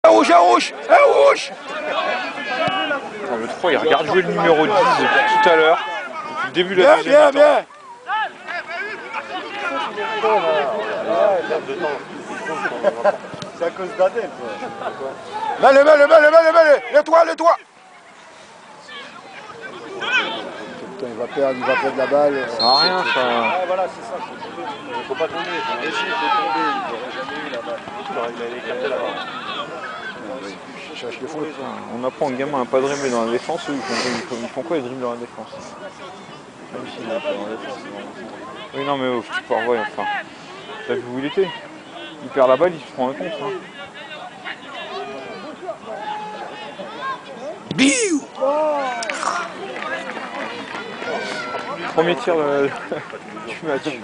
Guyancourt-Paris FC
6ème tour de la Coupe de France. 4 novembre 2007. Guyancourt (Excellence) vs Paris FC (National) : 0-1.